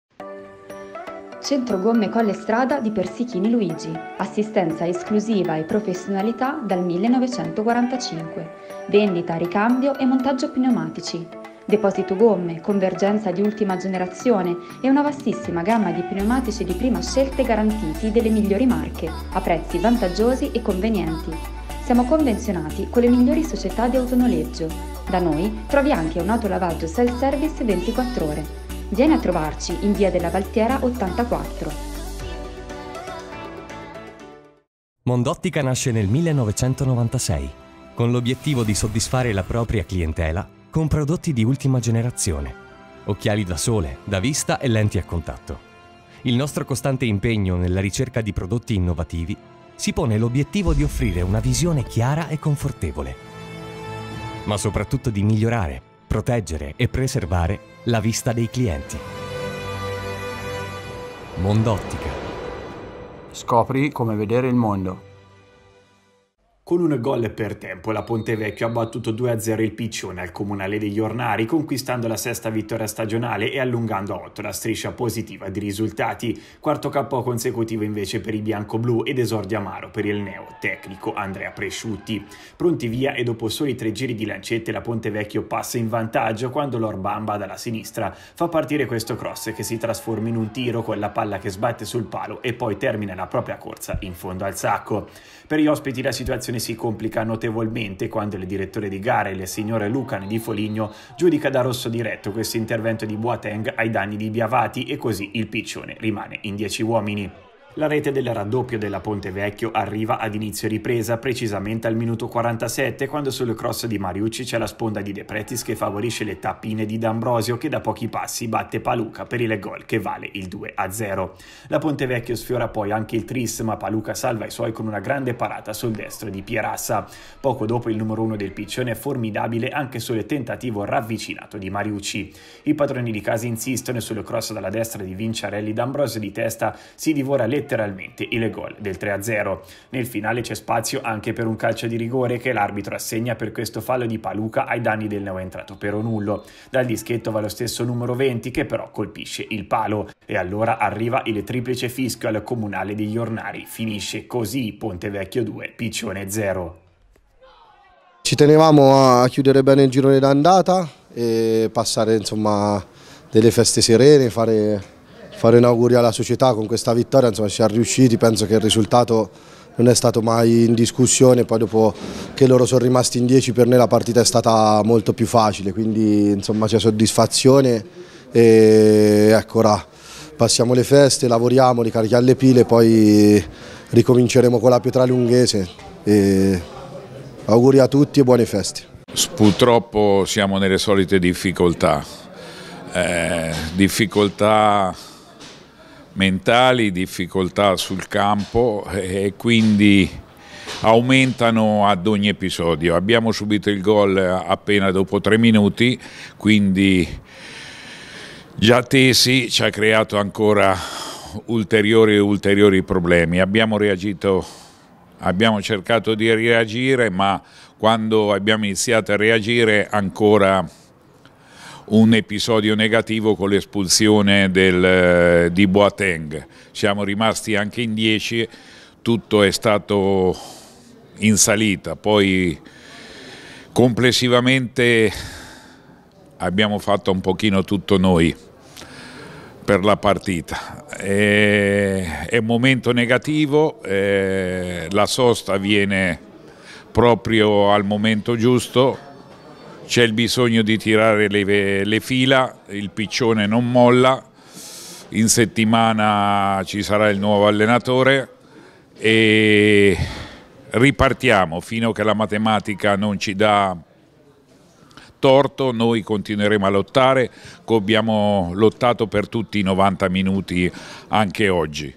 PONTEVECCHIO-PICCIONE 2-0 PROMOZIONE 17 dicembre 2023
Campionato di Promozione girone A - 15° giornata
Servizio di Alessio Romanelli